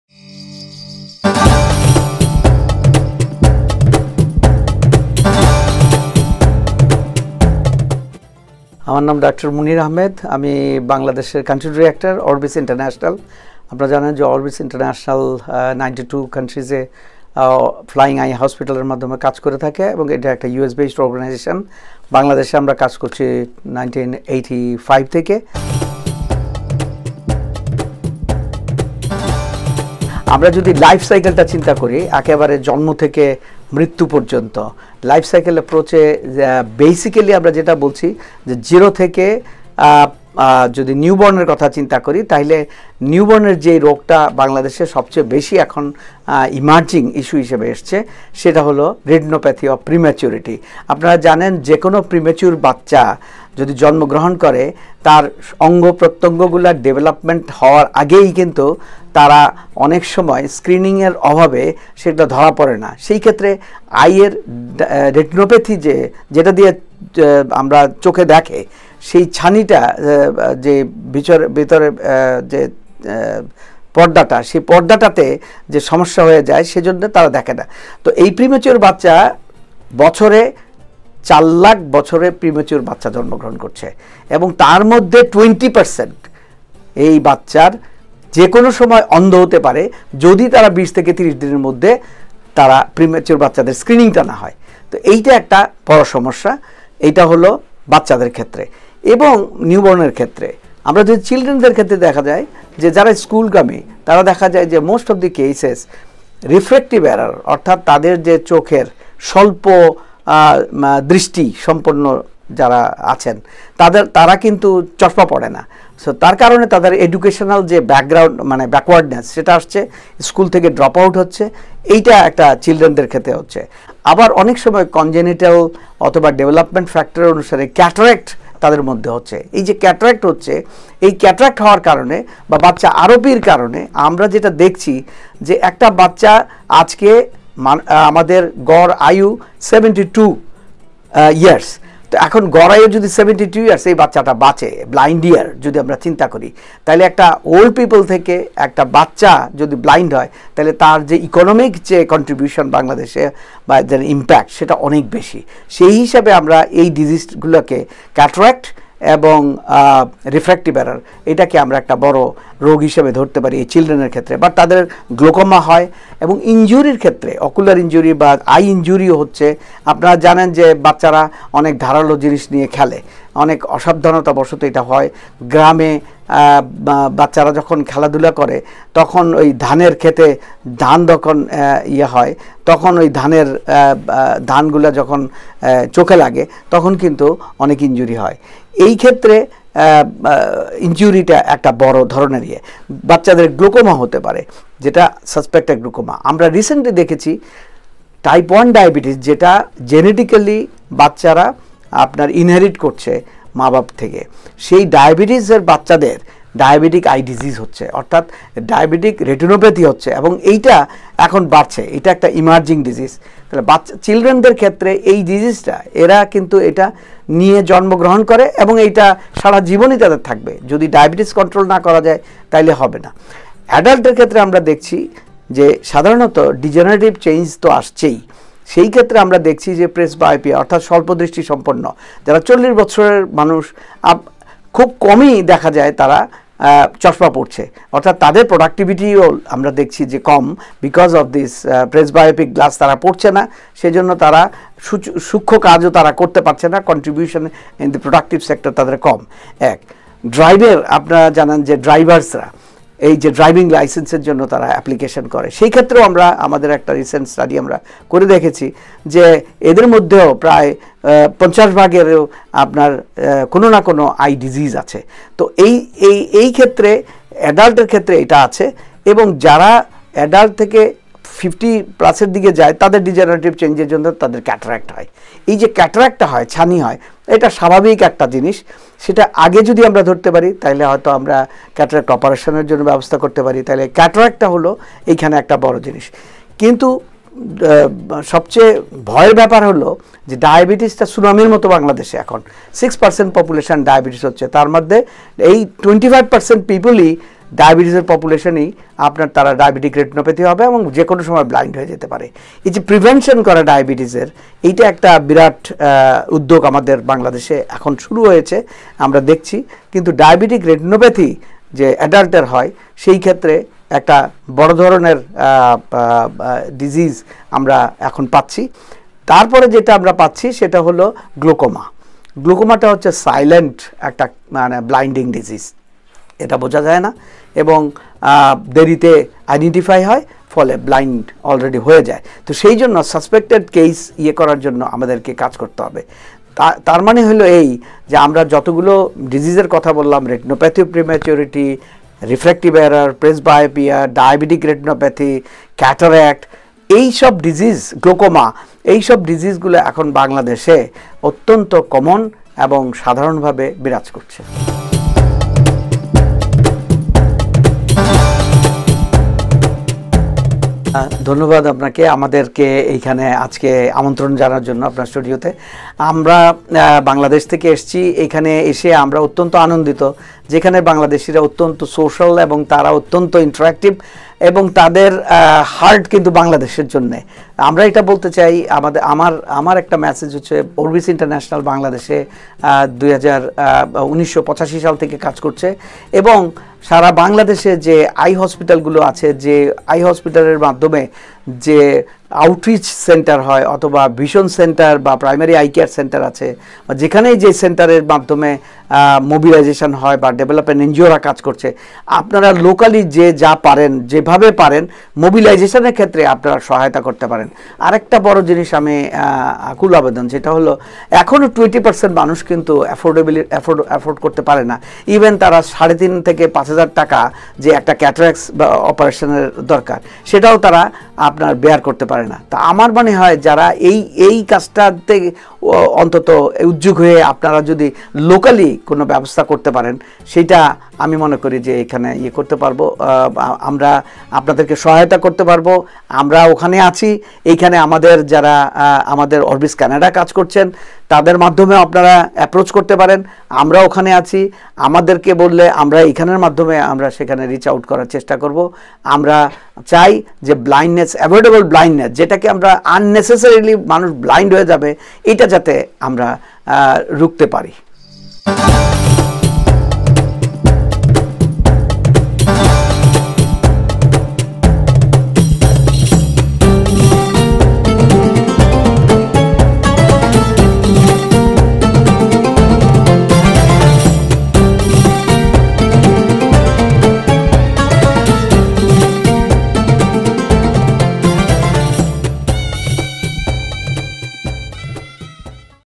Eye Care in Bangladesh
Dr Munir Ahmed, country director, ORBIS International Bangladesh program speaks with Radio Metro Mail, Canada about the eye care issues in Bangladesh.